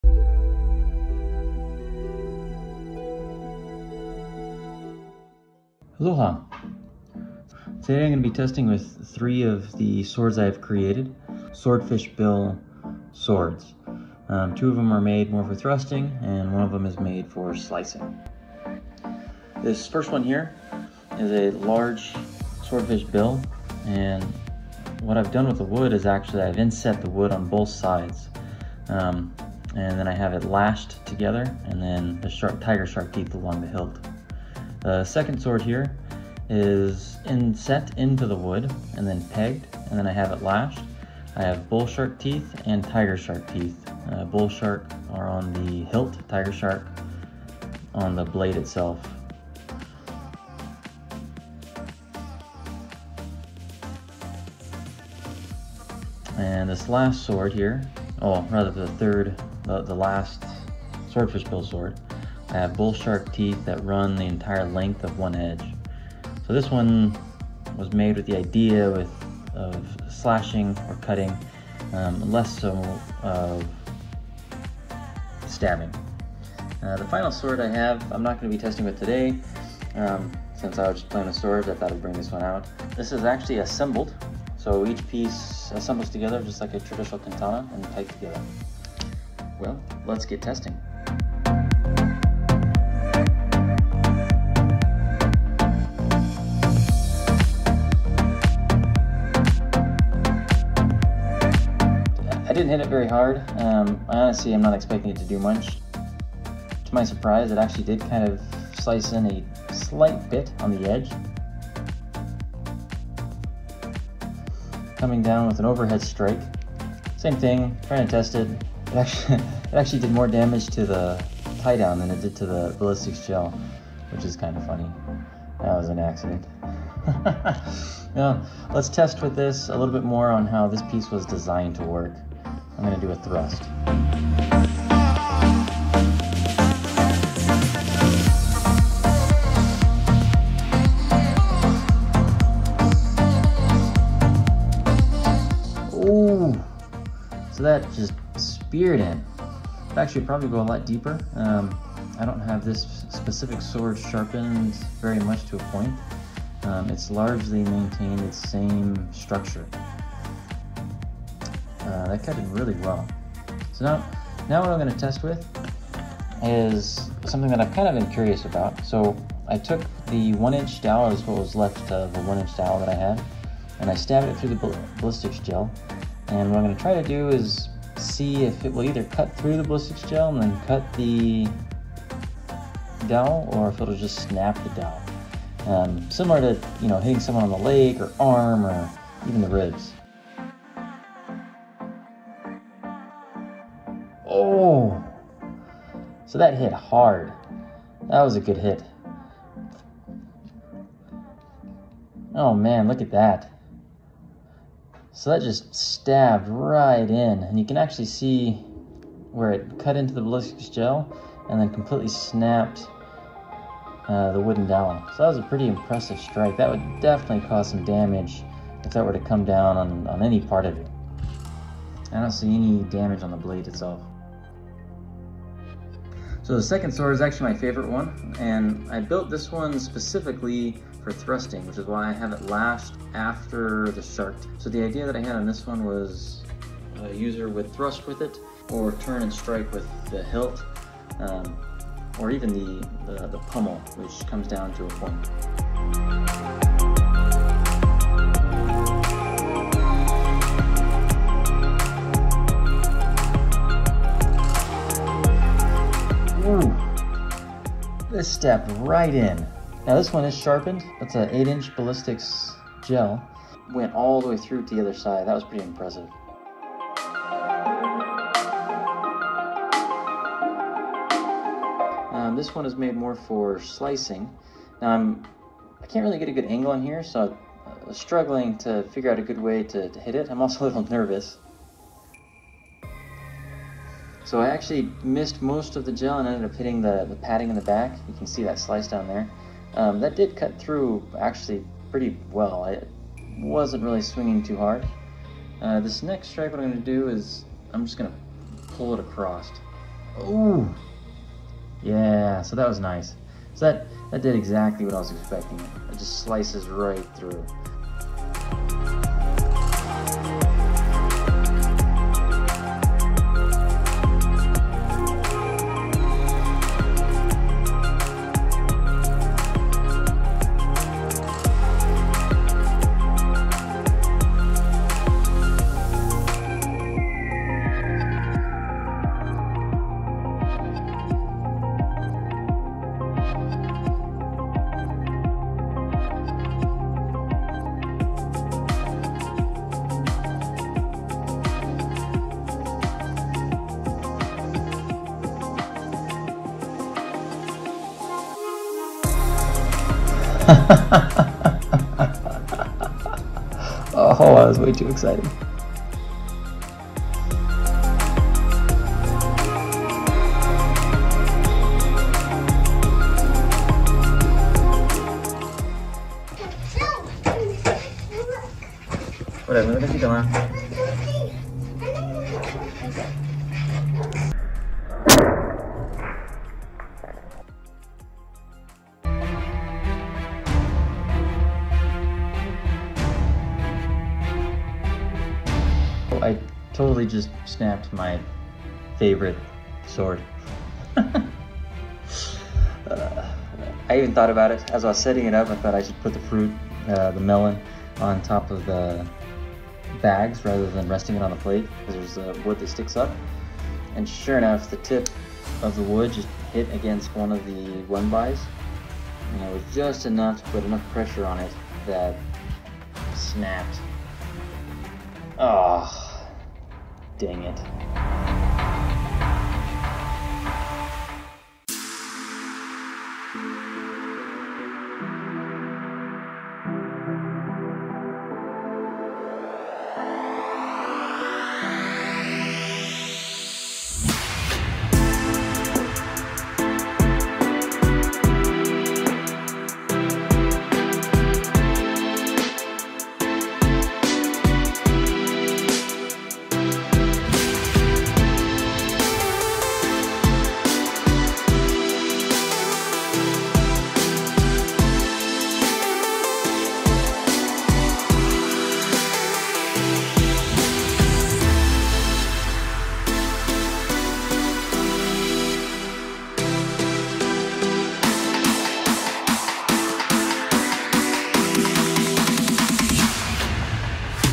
Function testing bone swords!!
Tested three different Hawaiian style swordfish bill swords on how they might have been used.